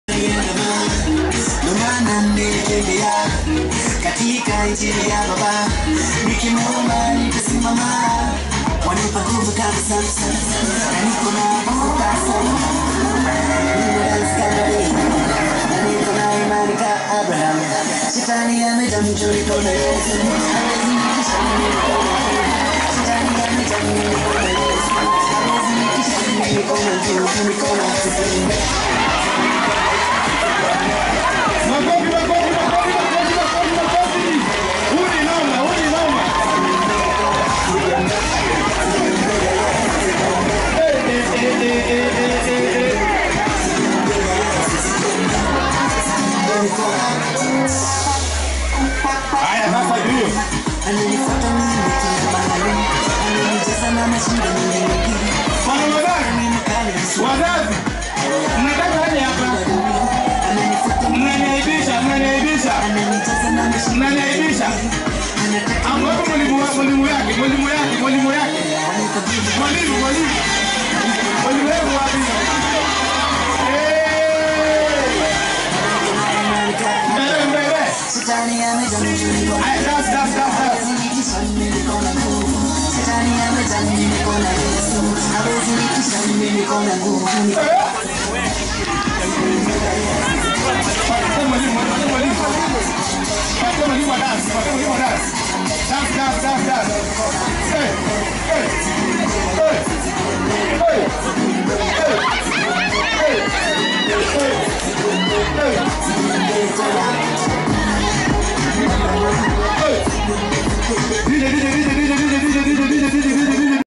LIGHT OF HOPE ACADEMY SCHOOL KISUMU,5TH GRADUATION CEREMONY TALENTS 25TH OCTOBER 2018
TALENTED YOUR GIRL DANCER